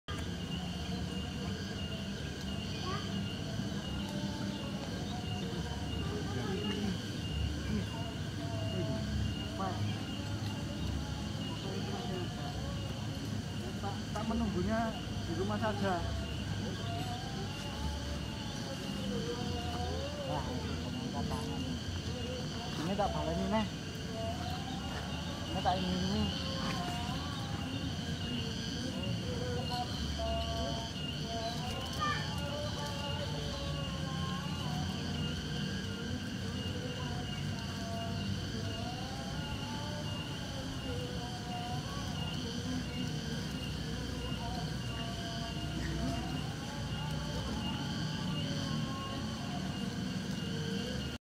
(background video) keindahan pemandangan alam di danau,
selain tempat untuk memancing,, waduk selorejo ngantang juga memiliki pemandangan alam yang sangat indah,,